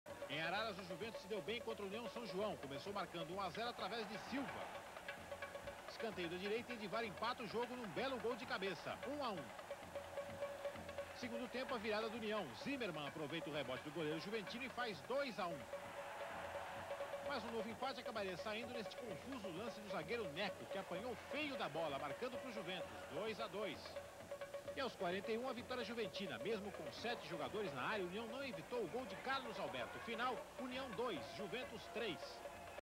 União São João 2 x 3 Juventus - Paulista A-1 1990
União São João 2 x 3 Juventus
Campeonato Paulista Primeira Divisão
Primeira Fase - Primeiro Turno - Grupo 01
Data: 21/02/1990
Local: Estádio Hermínio Ometto, Araras
Público: 1.250 pagantes
Renda: NC$ 62.500,00
Árbitro: Flávio de Carvalho
Auxiliares: Emílio Almeida Maciel e Hélio Ambrosio Filho
Cartões Amarelos: Marquinhos (USJ); Carlão e Albéri (JUV)
Cartão Vermelho: Paulo (USJ); Cossa (JUV)
Gols: União: Bafafá (34-1) e Zimmerman (5-2); Juventus: Silva (18-1), Neco (contra, 29-2) e Barbosa (41-2)
União São João: Pereira; Paulo, Fonseca, Djalma e Rossi; Marquinhos, Odair e Glauco; Kel (Neco), Bafafá (Zé Eduardo) e Zimmerman. Técnico: Palhinha.
Juventus: Cossa; Denílson, Albéri, Carlão e Donizete (Luis); Serjão, Carlos Alberto e Ricardo (Barbosa); Elcio, Claudinho e Silva. Técnico: Joel Castro Flores.

- União São João Esporte Clube